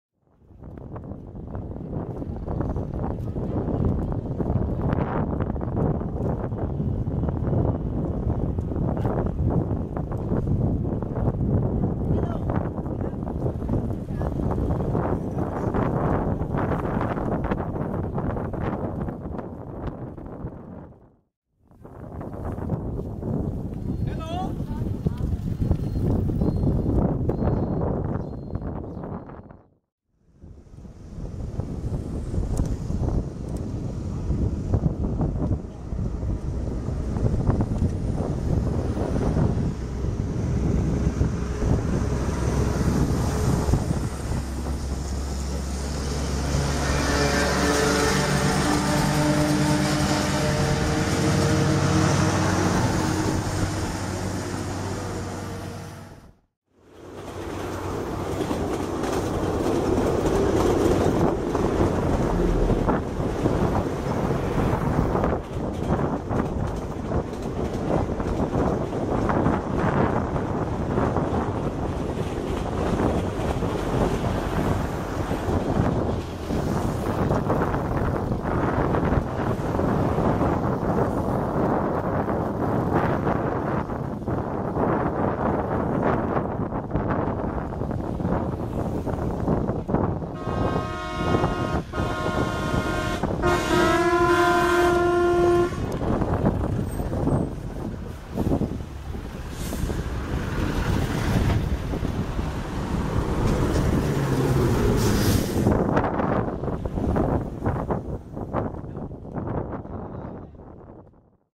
Vietnam, Phu Yen, Tuy An - highway scenery - Jan 2014
Cycling south along QL1A toward Tuy Hòa.
Starting at some side road with a road market selling bananas, passing a police checkpoint, then getting overtaken by the southbound train approaching Tuy Hòa. There is a lot of traffic because of people getting buses for Chinese New Year holidays (Tet).
Tuy An, Phú Yên, Vietnam.